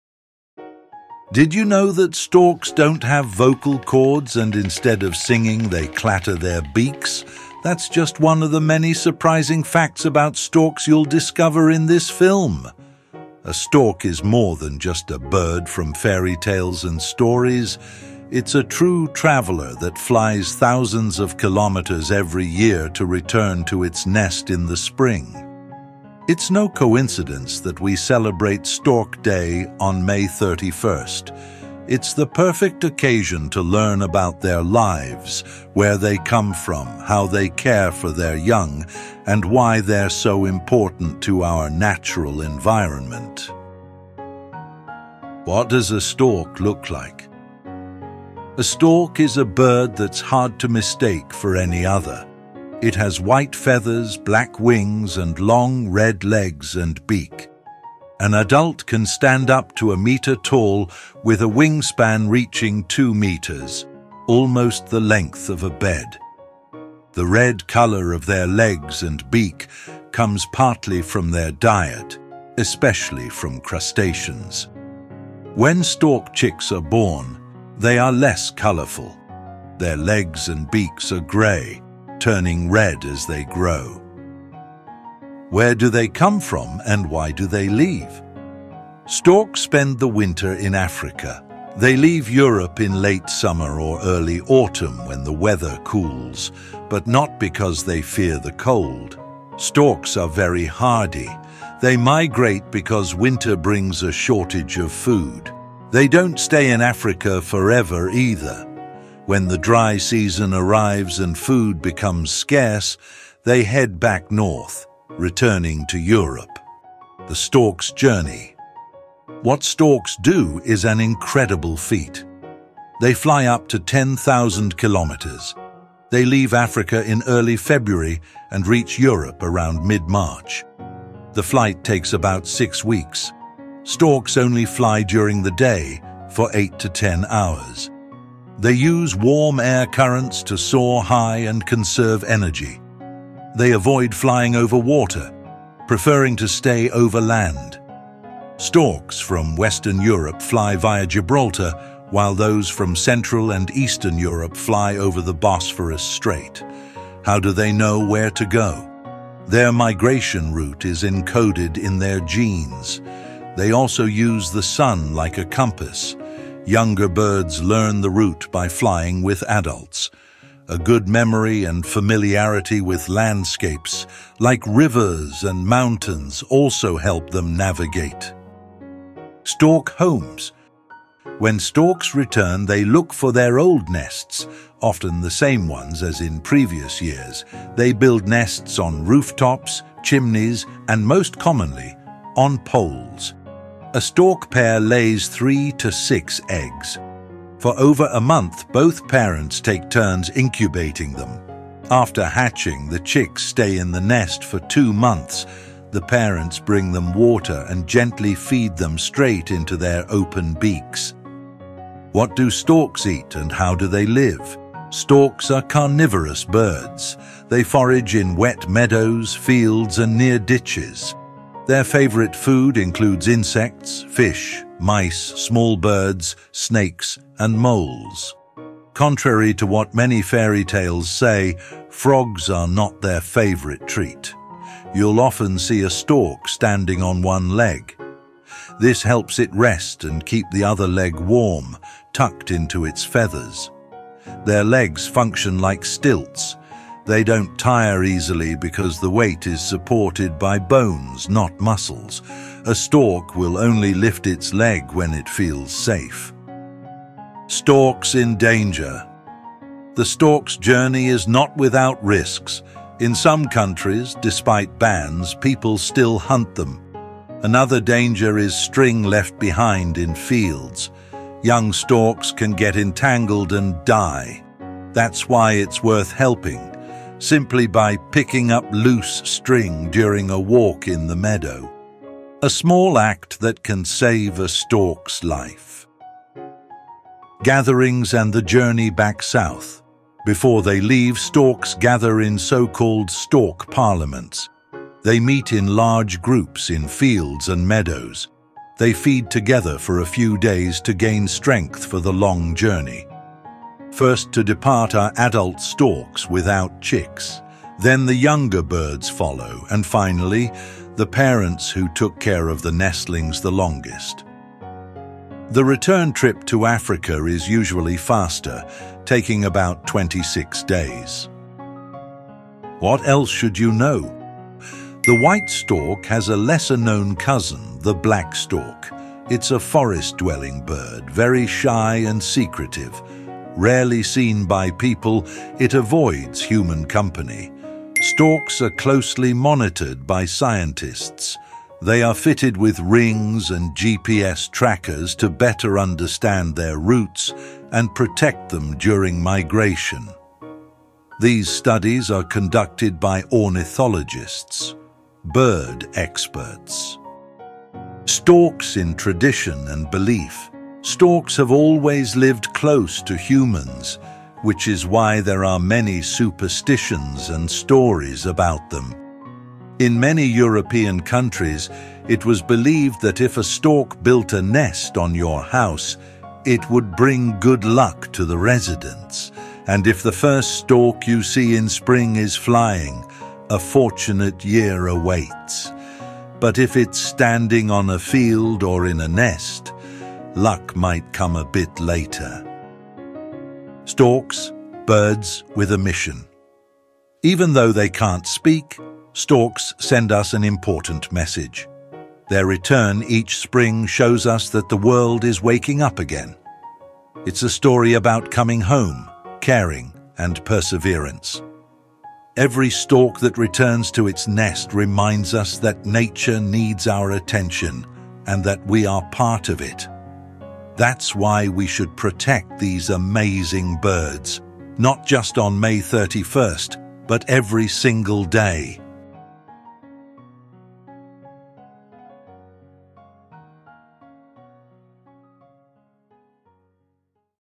How Do Storks Know Where to Fly?❓🌍 Educational Film About Storks | May 31 – Stork Day 🗓️
How Do Storks Know Where to Fly?❓🌍 Educational Film About Storks | May 31 – Stork Day - fascinating educational film, we uncover the secrets behind one of nature’s most impressive migratory journeys. Discover the wonders of bird migration as we follow the white stork – a majestic species known for its incredible seasonal travels between Europe and Africa.

Every spring and autumn, storks embark on epic journeys that span continents. But how do they know where to go? What guides them on their way? Do they rely on instinct, celestial navigation, magnetic fields, or perhaps something even more mysterious? This documentary brings you closer to the answers through captivating visuals, expert commentary, and a clear explanation of the science behind bird navigation.

Perfect for nature lovers, students, teachers, and anyone curious about wildlife, this film takes you into the world of storks like never before. You'll learn about:

The white stork's migration routes across Europe, the Middle East, and Africa.
The role of weather patterns, thermals, and wind currents in helping storks glide over vast distances.
How juvenile storks learn to migrate without guidance from their parents.
The impact of climate change and habitat loss on stork populations.
Conservation efforts across Europe to protect this iconic bird.

Celebrated across many countries, May 31 is Stork Day – a time to raise awareness about these elegant birds and the challenges they face in a rapidly changing world. Our video is the perfect way to celebrate this day with knowledge and appreciation for one of Europe’s most symbolic species.

🧭 Whether you're watching for fun or education, this documentary offers a unique opportunity to learn about one of the most extraordinary natural phenomena on Earth.

📅 Watch now and celebrate Stork Day with us – May 31!